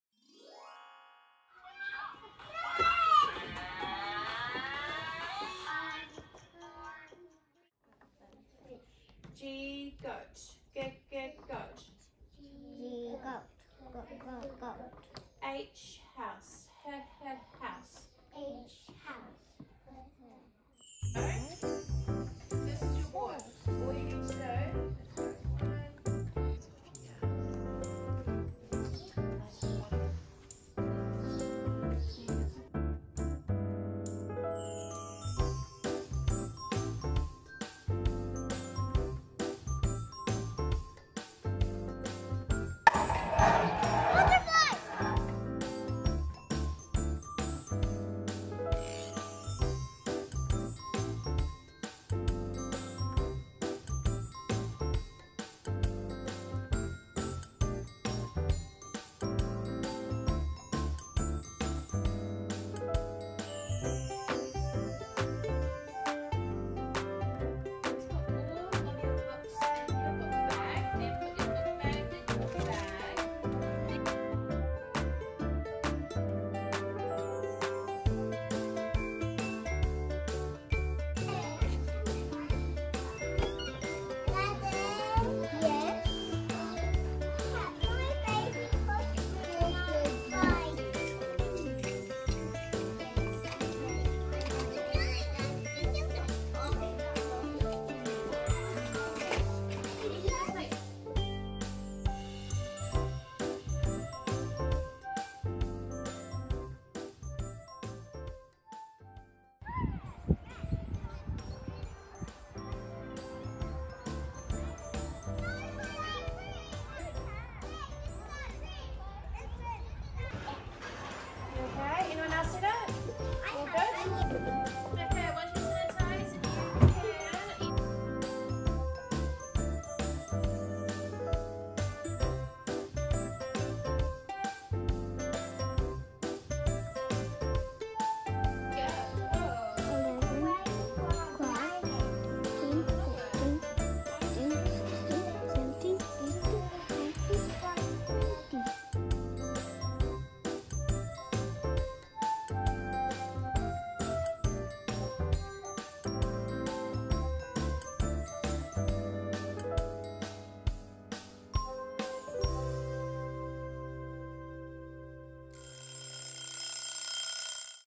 A day in the life of Year 0s at Glendowie School
Do you want to join us on a sneak peak into the lives of our year zeros?
Whatever we do, from playing to learning, we do everything to the highest standard, here at Glendowie School.
Watch as we take you through their daily schedule, and a quick little tour of their own, around our school.